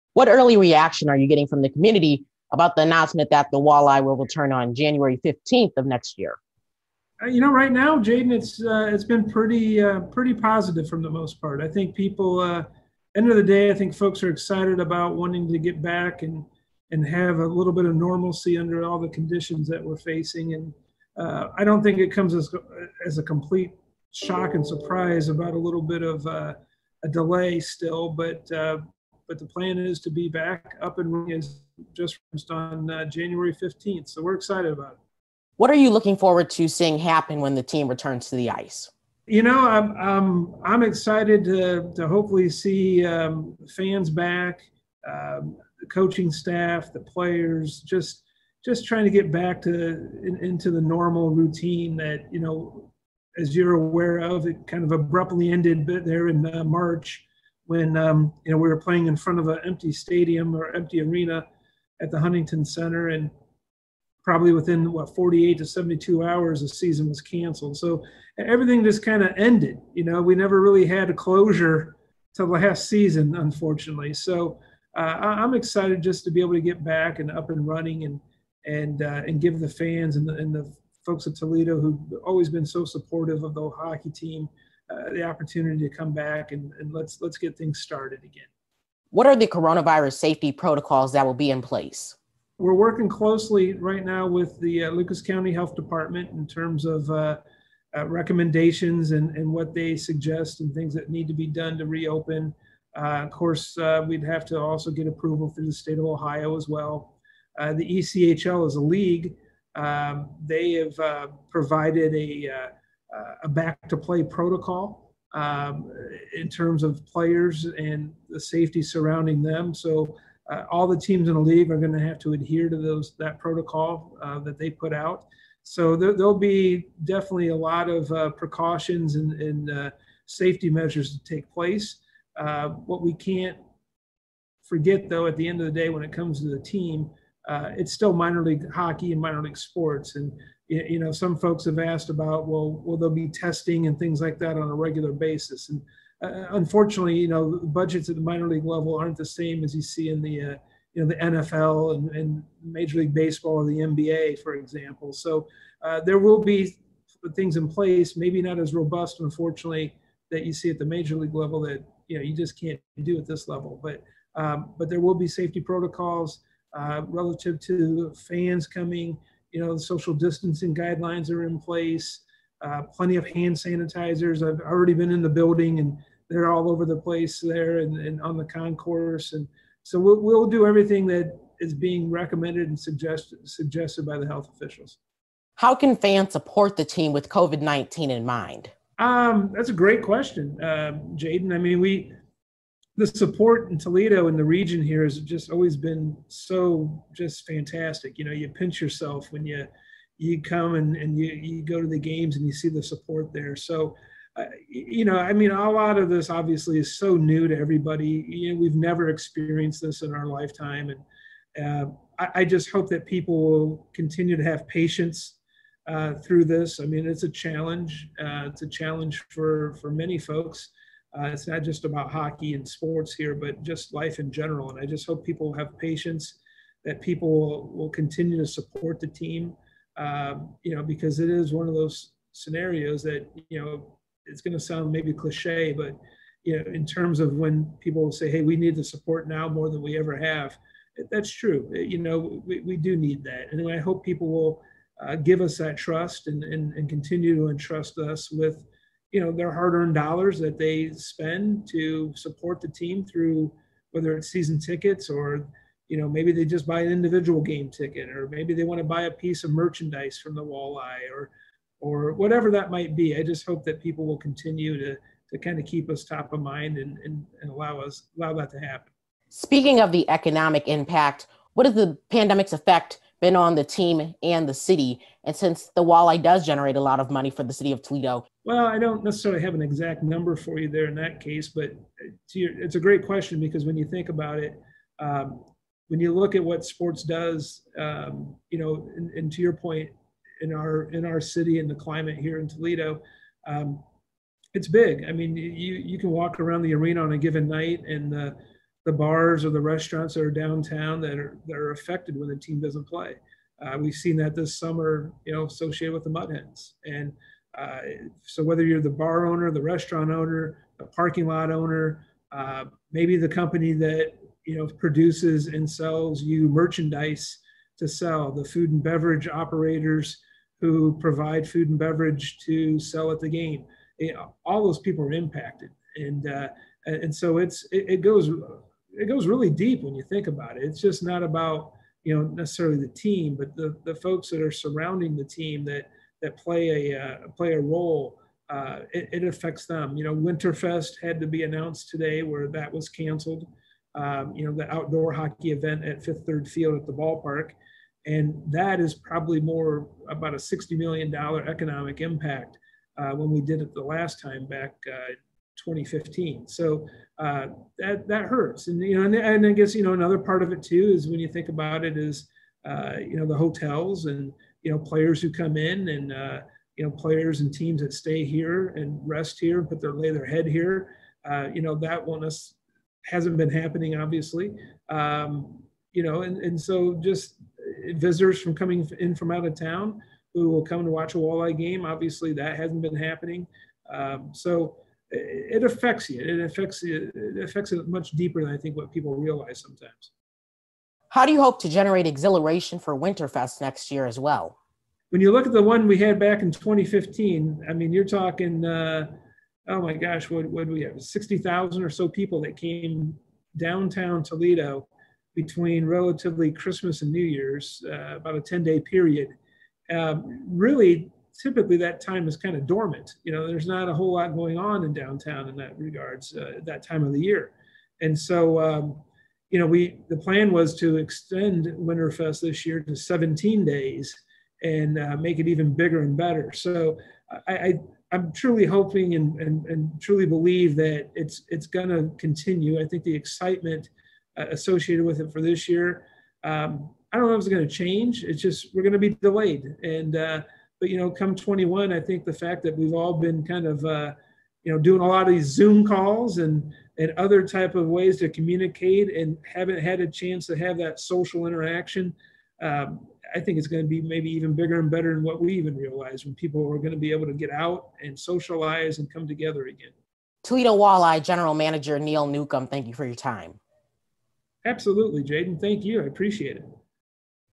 One-On-One with Toledo Walleye General Manager Neil Neukam
TOLEDO, Ohio — Friday, the Toledo Walleye announced their return to the ice on January 15 of next year. Here's what the Walleye's General Manager Neil Neukam has to say about coronavirus safety precautions, as well as the community response to the exciting news.

"...it's been pretty positive for the most part, at the end of the day I think folks are excited about wanting to get back and have a little bit of normalcy under all the conditions that we're facing. I don't think it comes as a complete shock and surprise about a little bit of a delay still..." says Walleye General Manager Neil Neukam.